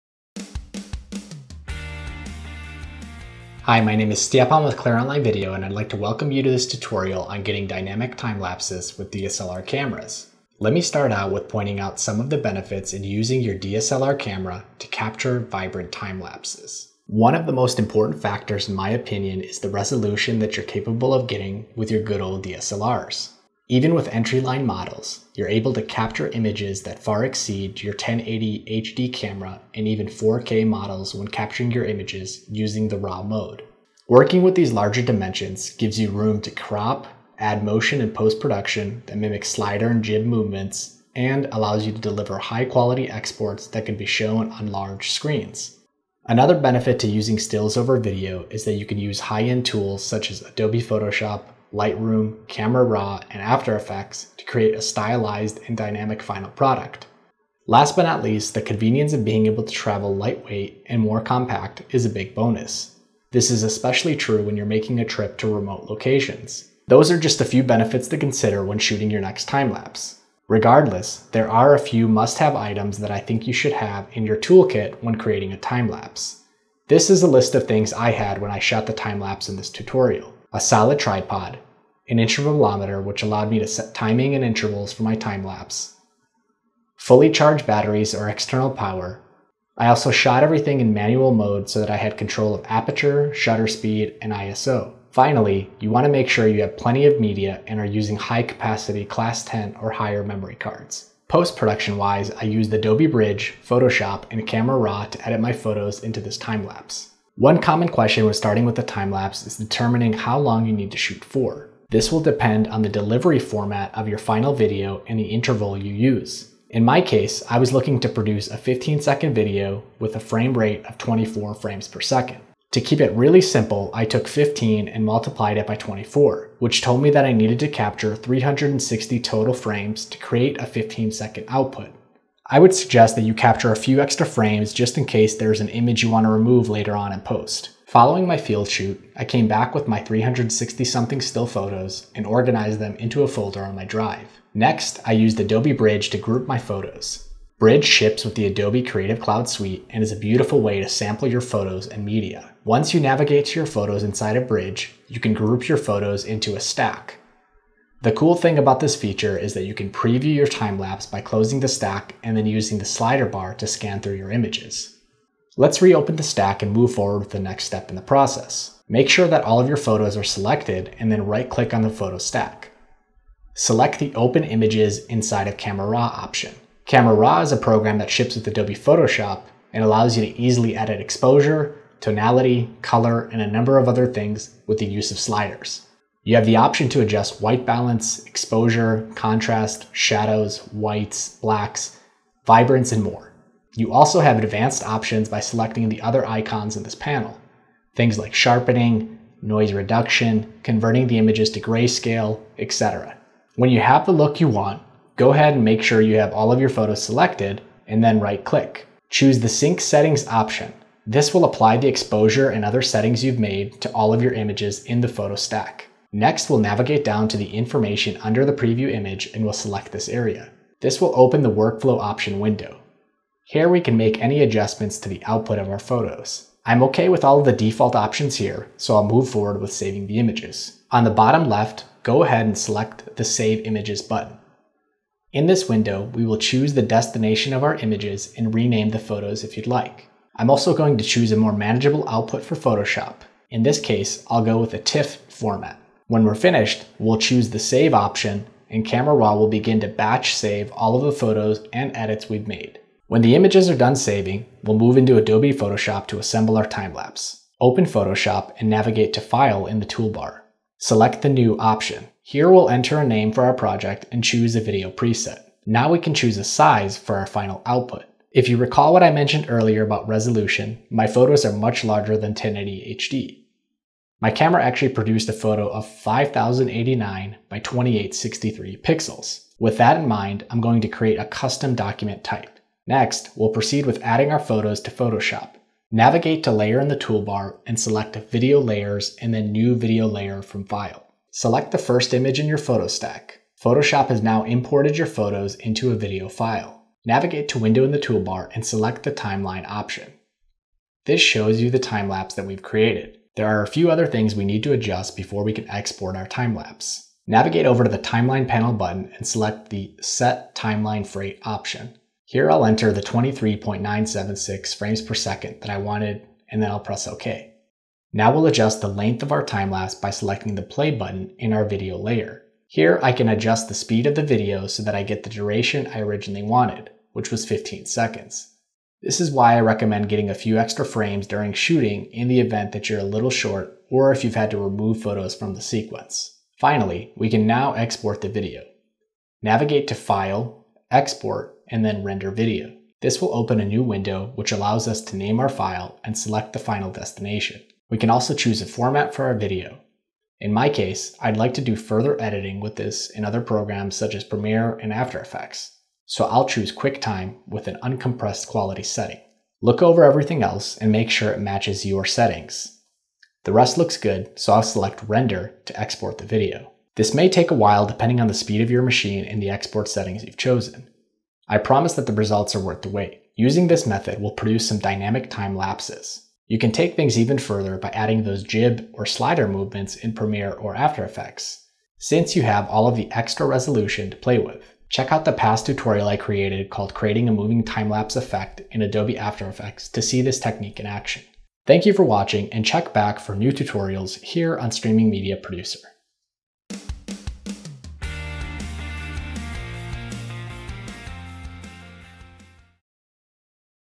How to Create Vibrant Timelapses With DSLRs
Stjepan Alaupovic of Clear Online Video demonstrates how to create vibrant timelapses using high-resolution images captured with DSLRs, and edited and exported with Adobe Bridge, Camera Raw, and Photoshop.